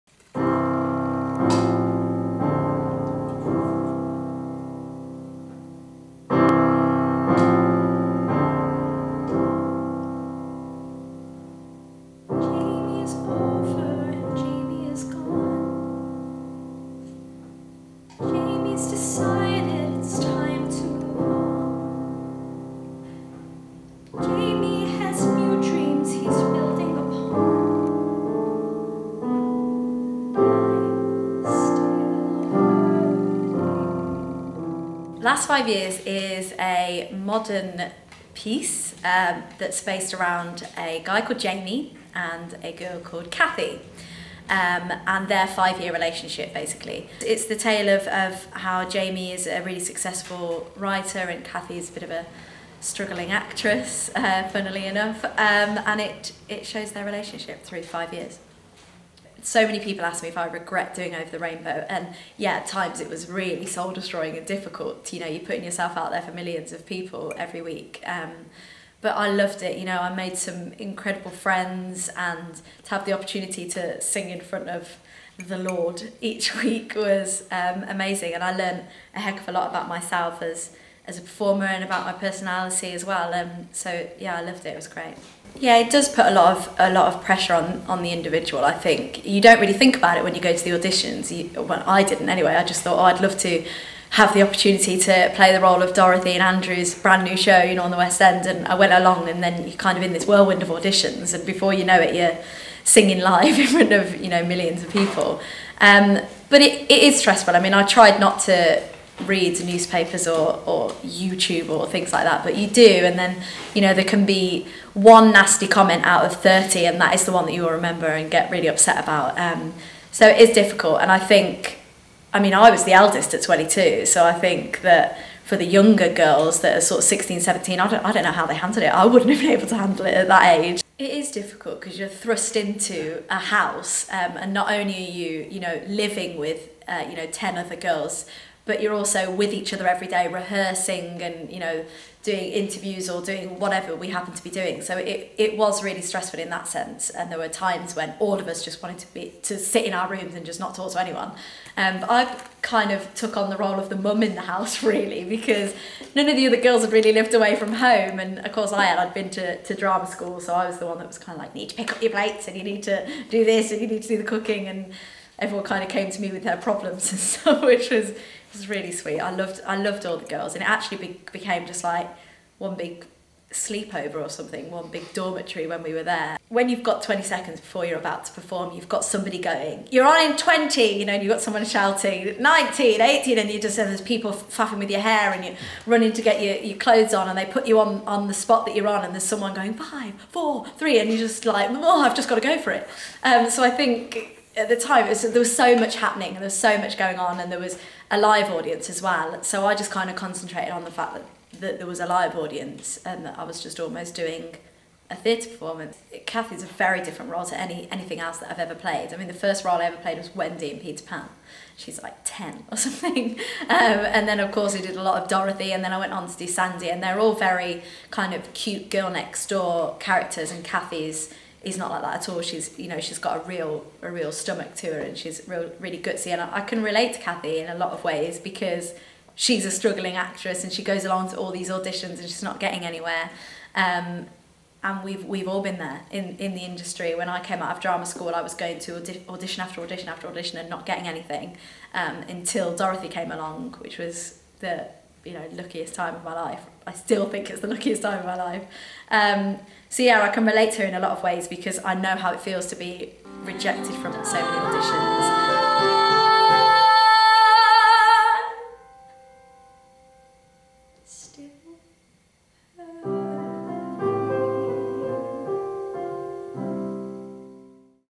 Lauren Samuels Interview - The Last Five Years - The Tabard Theatre
For more information and to book tickets for The Last Five Years go

The Last Five Years is a show about a marriage. It is told from two angles: from the man s point of view it starts at their first meeting and ends in the present, when the marriage breaks up. From the woman s point of view, it starts in the present and works back in time to the first meeting. Only at the wedding, in the centre of the piece, do the stories intersect. The relationship is between a 'nice jewish boy and an Christian girl in New York City. During a five year period, they fall in love, marry and their relationship gradually sours and they finally divorce.